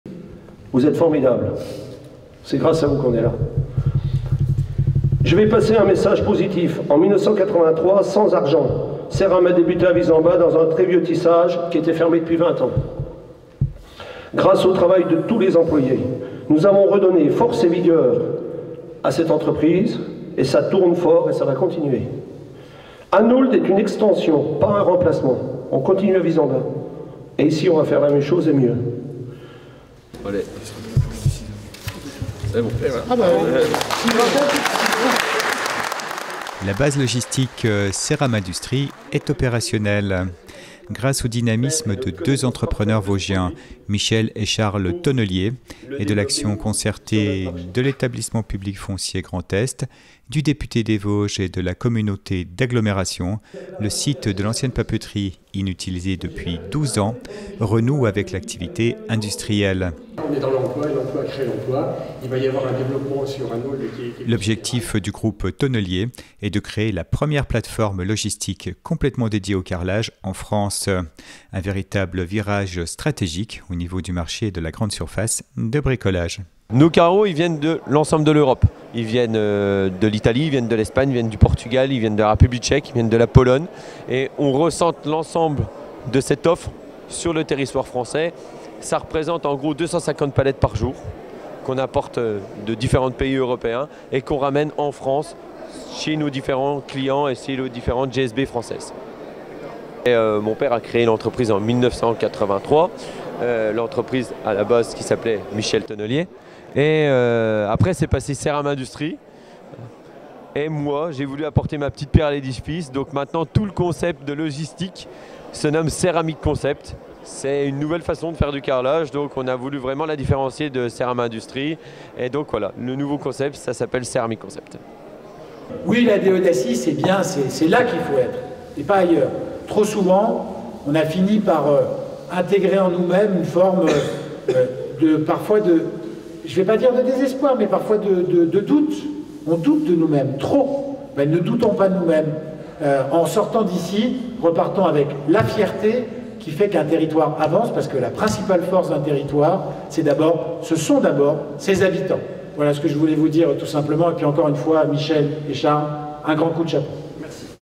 La base logistique Ceram Industrie, groupe Thonnelier, opérationnelle à Anould !
La base logistique Ceram industrie est opérationnelle  !
Grâce aux dynamisme de deux entrepreneurs vosgiens Michel et Charles Thonnelier et de l'action concertée de l'Etablissement Public Foncier Grand Est, du député des Vosges et de la Communauté d'Agglomération , le site de l'ancienne papeterie, inutilisé depuis 12 ans,  renoue avec l'activité industrielle !
L'objectif du groupe Thonnelier est de créer la première plate forme logistique complètement dédiée au carrelage en France, un véritable virage stratégique au niveau du marché de la Grande Surface de Bricolage .
Les carreaux viennent de toute l'Europe Italie, Espagne Portugal, République Tchèque, pologne...
Cela représente 250 palettes par jour
500 références de carrelage seront stockées dans l'entrepôt avec des services innovants : préparation des commandes en 12 h et livraison en 72 h. Le siège de la société , fondée par Michel Thonnelier, restera implanté à Wissembach

Cette installation sur une friche existante permet également d'éviter la construction de nouveaux bâtiments sur des zones agricoles !